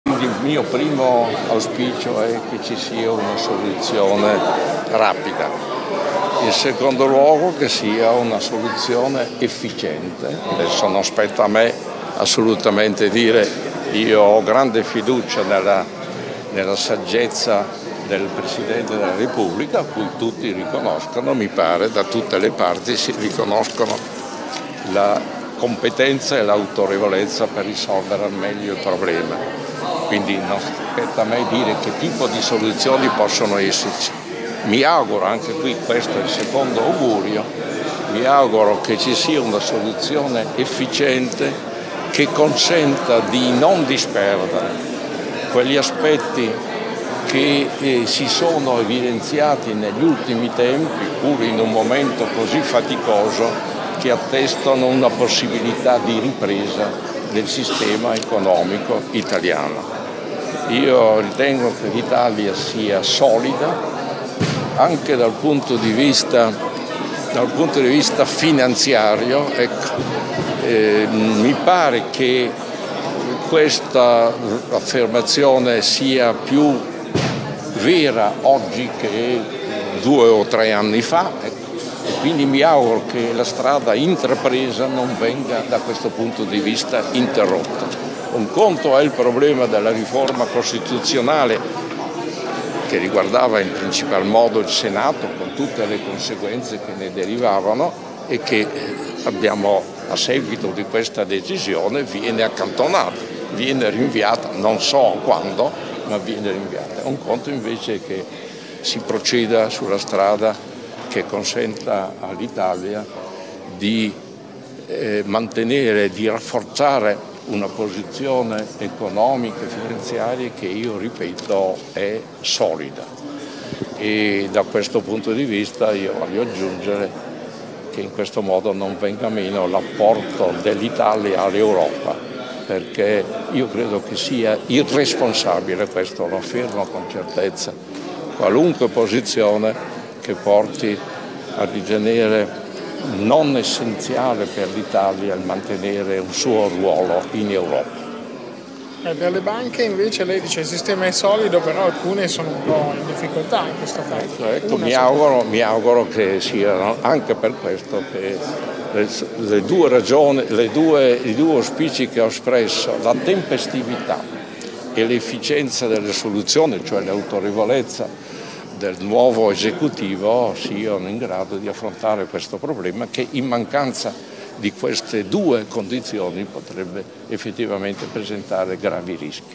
Bazoli: “Senza tempestività del nuovo esecutivo le banche rischiano”
ECONOMIA (Milano). "Spero ci sia una soluzione rapida ed efficiente. Ho grande fiducia nel presidente della Repubblica. L'Italia è solida anche dal punto di vista finanziario. Se non ci saranno tempestività e autorevolezza del nuovo esecutivo potrebbero esserci rischi per le banche. E' fondamentale che chiunque salga al Governo capisca l'importanza dell'Europa per l'Italia". Queste le parole di Giovanni Bazoli, presidente emerito di Intesa Sanpaolo, presente stamani a Palazzo Marino per la presentazione della mostra 'Piero della Francesca. La madonna della Misericordia', che ha commentato la vittoria del No al referendum e il possibile impatto sui mercati e sulle banche. (Carmen la Gatta/alaNEWS)